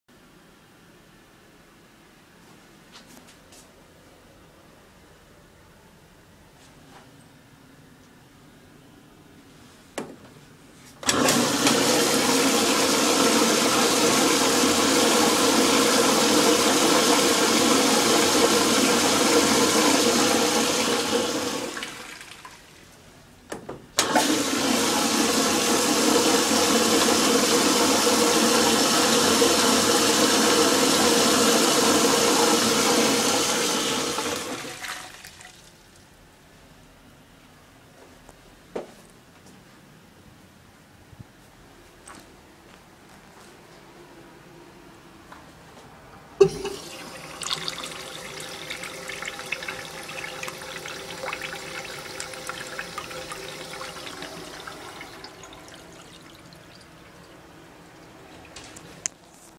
Unkown Urinals and PRESTO Button Flushometers on a PORCHER Bowl
Filmed in Marseille in France.
A guy saw me with my camcorder, this was very embarassing lol, this is why I do not film the Urinals that often, I'm not a risk taker.